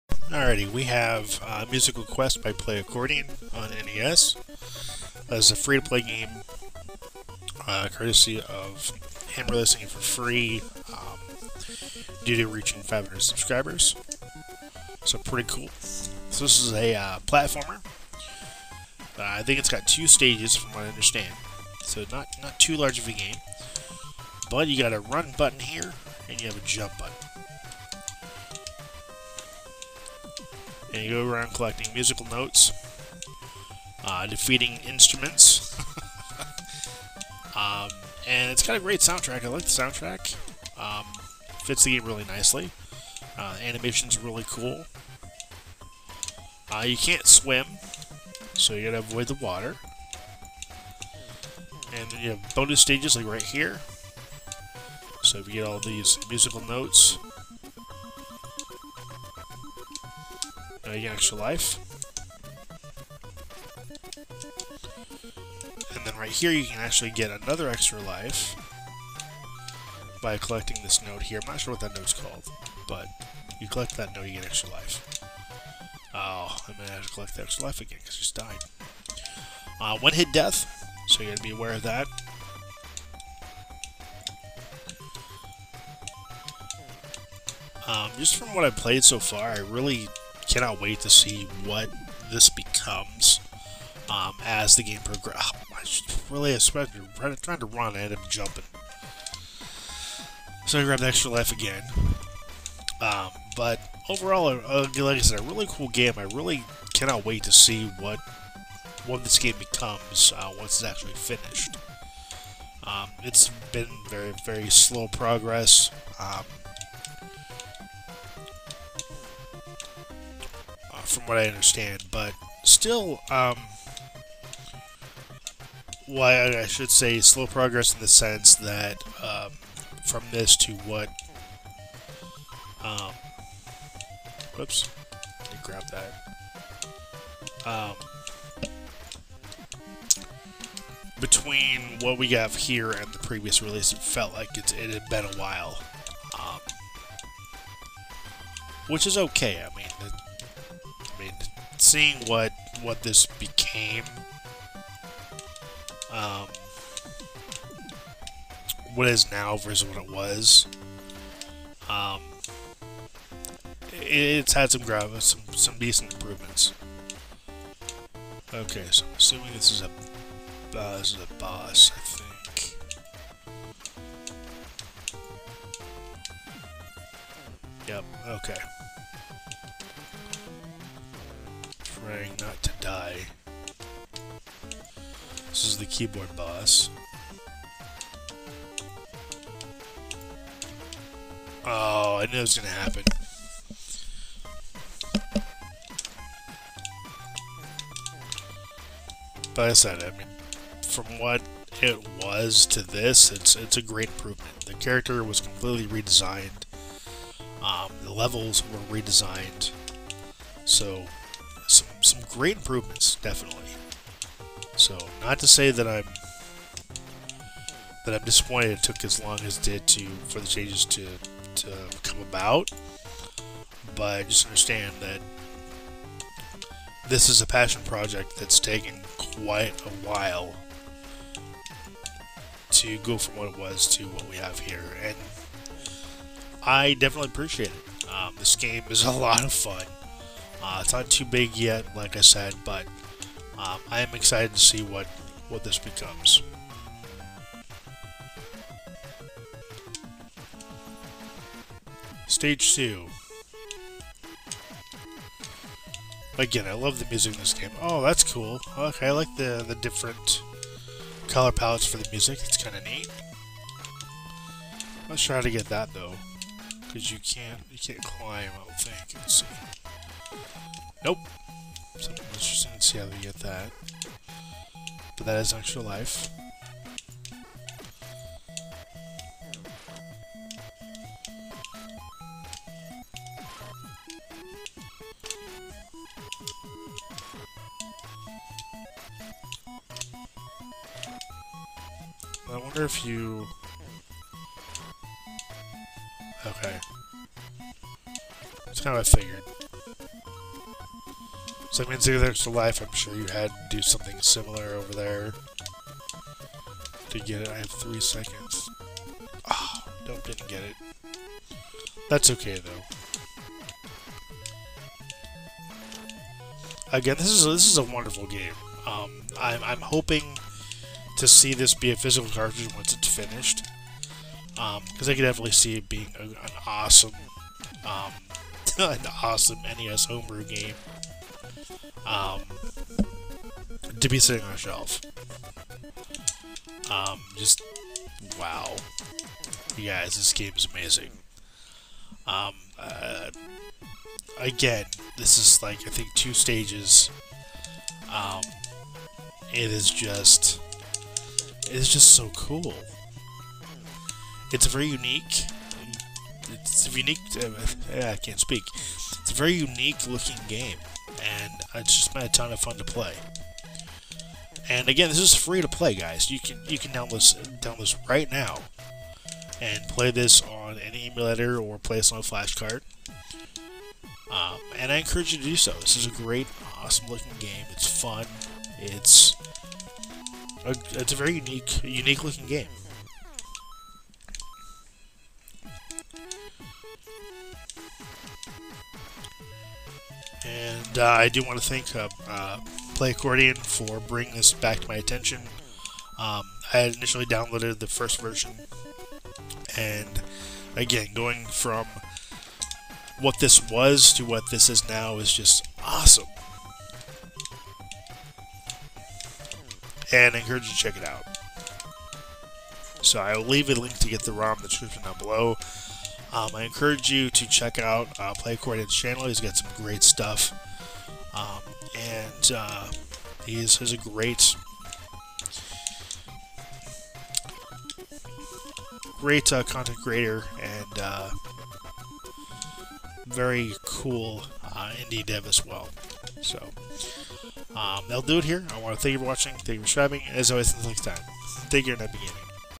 PlayAccordion - Musical Quest NES Review
PlayAccordion - Musical Quest is a short but awesome platforming game for the NES from m8si.

The game is available as a ROM and you can get it free